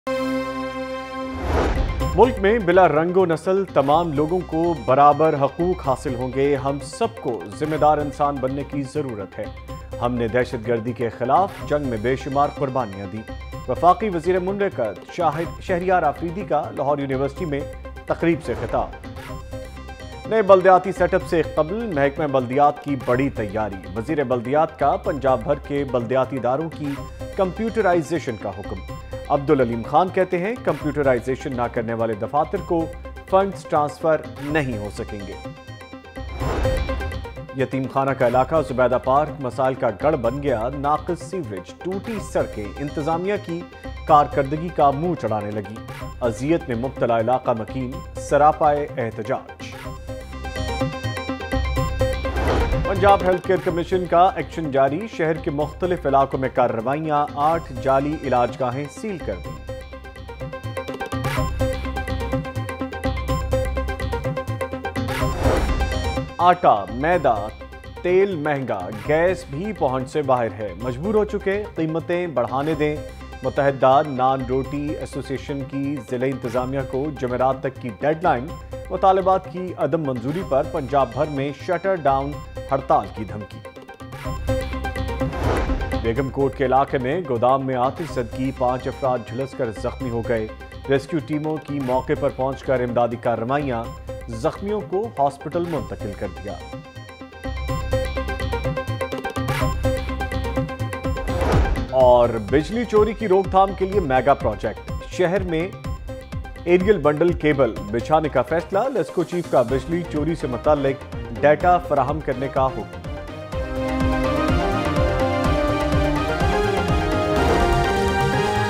11 PM Headlines Lahore News HD – 26th November 2018
Watch Latest News: Khawaja Saad Rafique bail extended till 5th December in Paragon Housing Scandal, Husband commits suicide after killing wife and son in Lahore, LESCO announces mega project to terminate electricity theft,  Attack at Chines consulate Karachi, PSCA begins challan on over speeding, NAB Court approves 7-days transit remand of Shehbaz Sharif.  Shehbaz Sharif to appear before NAB Court in Aashiyana Iqbal Scandal, Nation celebrates Eid Milad un Nabi, Roads and streets decorated on Eid Milad un Nabi, CJP orders larger bench  to constitute JIT on Model Town tragedyPTI wins two senate seats, 6 Dead in truck accident near Sundar, Pakistani actor Asad Malik apprehended for carrying weapons at Lahore airport, Nawaz Sharif and Khaqan Abbasi to appear before LHC in treason case, IG Punjab assures to de-politicize police, Lahore among worst polluted cities ,Gas shortage prevails in Lahore, , Gas load shedding starts in different areas across city, Petrol prices witness sharp increase, Dolphin Force officer opens fire at local, Poultry prices witness sharp increase, Dry Fruit prices increases, , Immense traffic jam in Lahore, Scuffle breaks out between locals and traffic wardens, Dollar prices witness sharp increase, Oil tanker collides at Waris Meer underpass and much more.